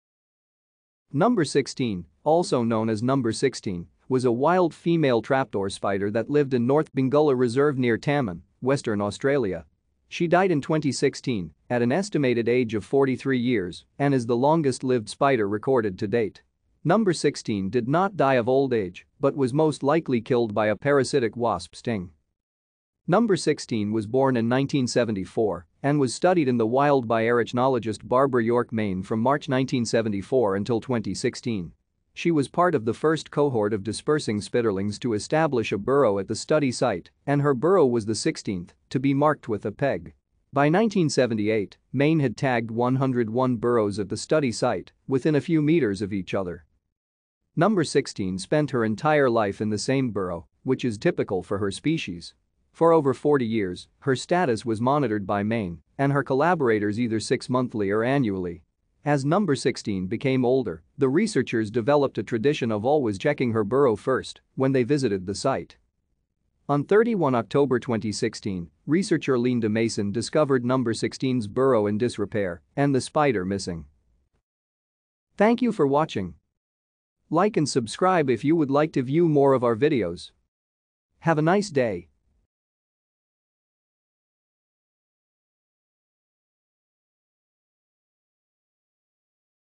Number 16 (Spider)
Number 16, also known as #16, was a wild female trapdoor spider  that lived in North Bungulla Reserve near Tammin, Western Australia. She died in 2016, at an estimated age of 43 years, and is the longest-lived spider recorded to date. Number 16 did not die of old age, but was most likely killed by a parasitic wasp sting.